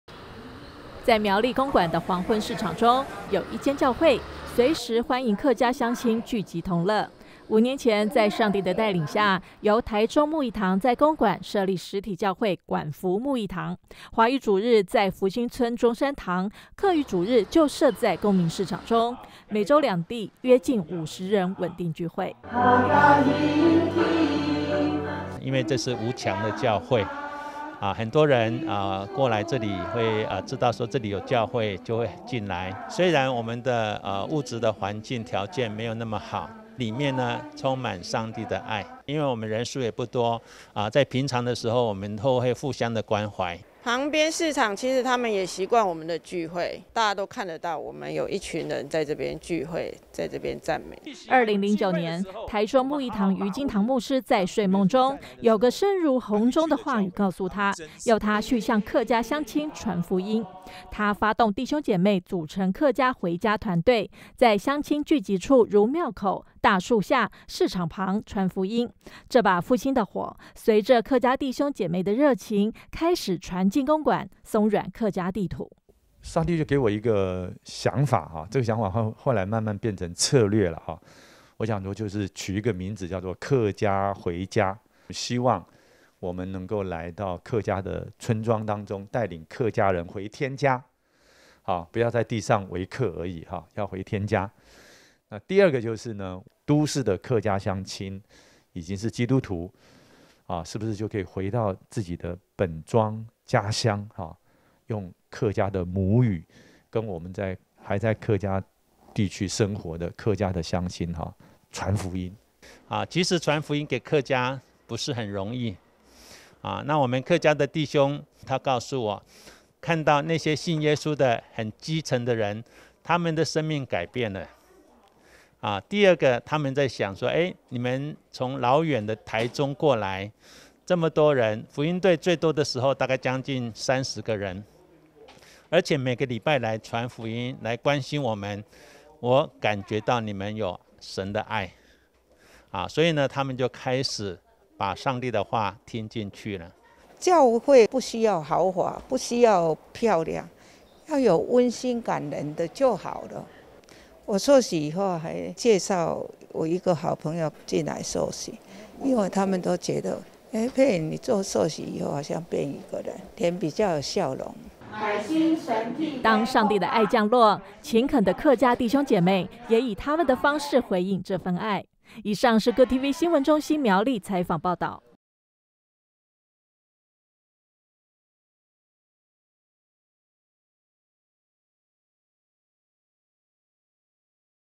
上帝的愛降落 苗栗客家黃昏市場中的教會-GOOD TV NEWS
華人教會國度新聞盡在GOOD TV

在苗栗公館的黃昏市場中，有一間教會，隨時歡迎客家鄉親聚集同樂。5年前在上帝的帶領下，由台中慕義堂在公館設立實體教會館福慕義堂，華語主日在福星村中山堂，客語主日就設在公明市場中，每週兩地約近50人穩定聚會。
2009年，台中慕義堂于金堂牧師在睡夢中，有個聲如洪鐘的話語告訴他，要他去向客家鄉親傳福音，他發動弟兄姊妹組成客家回家團隊，在鄉親聚集處如廟口、大樹下、市場旁傳福音，這把復興的火，隨著客家弟兄姊妹的熱情，開始傳進公館，鬆軟客家地土。
當上帝的愛降落，勤懇的客家弟兄姊妹，也以他們的方式，回應這份愛。